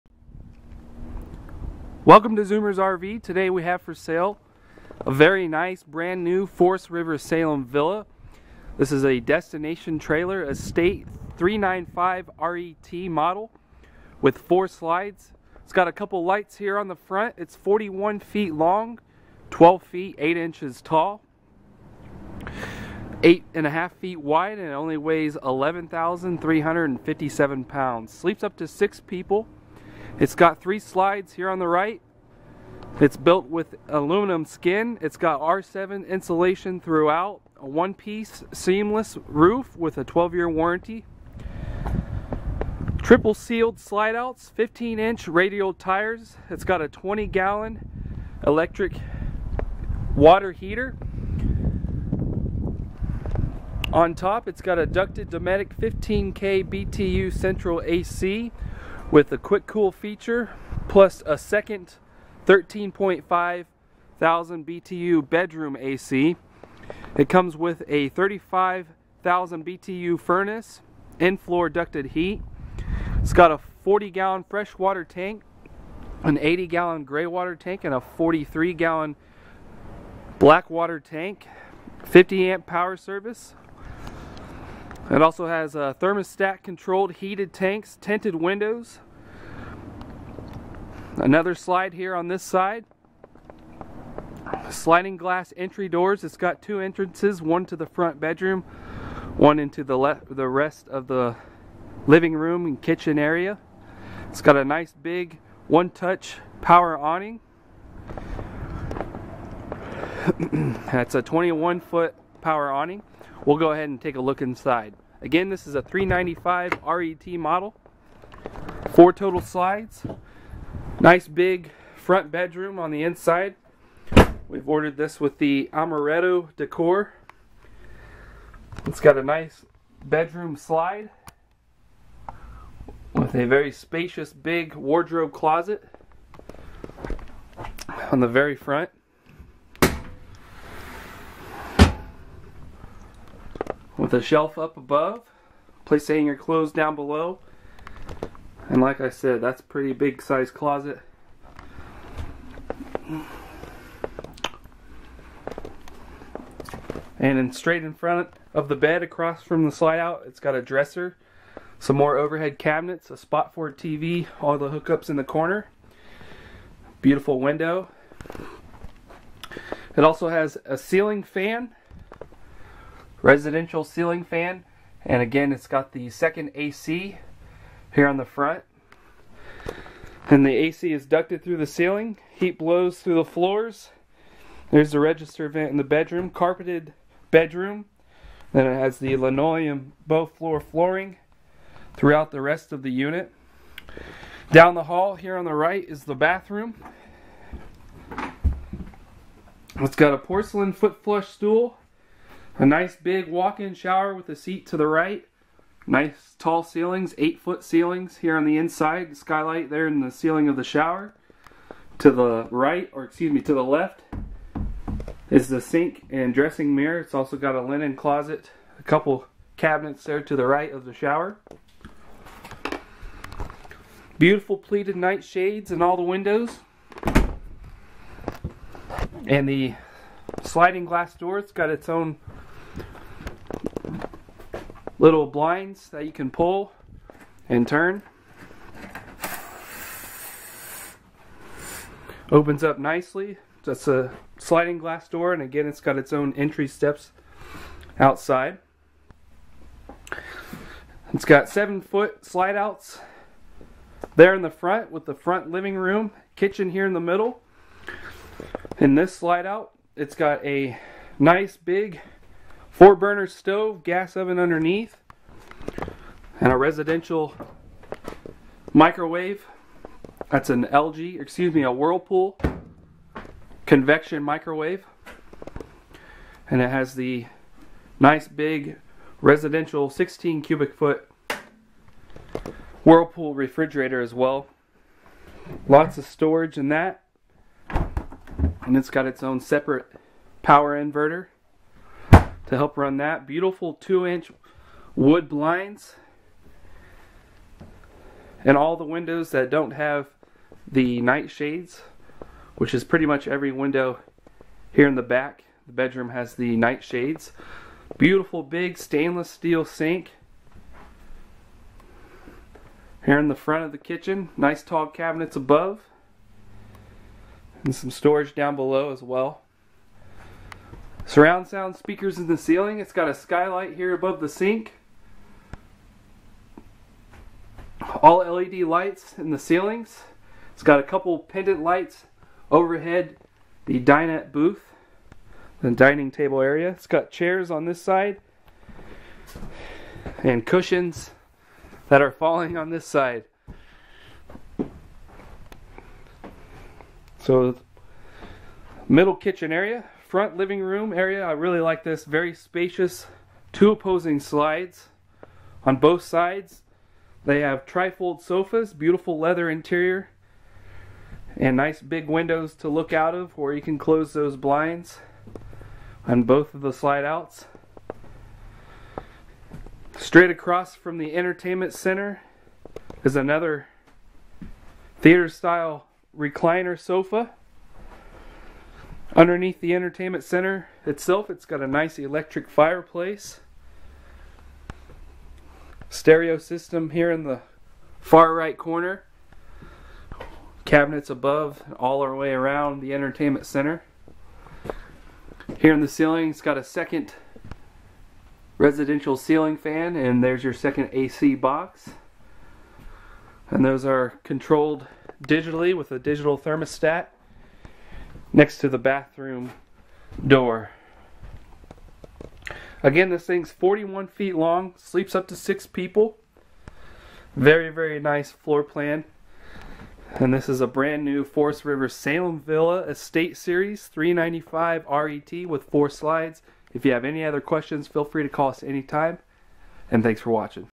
2017 Forest River Salem Villa Estate 395RET
Awesome New 41' 2017 Forest River Salem Villa Estate 395RET Destination Trailer 4-Slides!! This Beautiful High Quality Quad Slide Camper by Forest River comes Fully Loaded.. Sleeps 6, Spacious Rear Living Room With Two Opposing Slides and a Leather Tri-Fold Sofa In Each, Rear Entertainment Center and Fireplace, Surround Sound Speakers, Theater Seating, Big Kitchen Slide and Dinette Accross, Private Queen Bed Slide up Front with 2nd Outside Door and Big Front Wardrobe Closet! This Thing Also Has Tons Of Features Like Two Roof Airs, In-floor Ducted 35,000 BTU Furnace, Power Awning w/ Multi-Colored LED Light Strip, Outside Speakers, Residential Exterior Lighting on the Front, Middle Sliding Glass Door Entrance, Central Vac System, 15" Nitrogen Filled Radial Tires and More!! Come Check Out This Brand New Destination Trailer Today!! MSRP $55,404 ZOOMERS SUPER LOW PRICE IS $38,250 MONTHLY PAYMENT $331 NO MONEY DOWN with approved credit.. Sales Tax Included
Questions about the RV Call
Office Number: 1-877-8-ZOOMER or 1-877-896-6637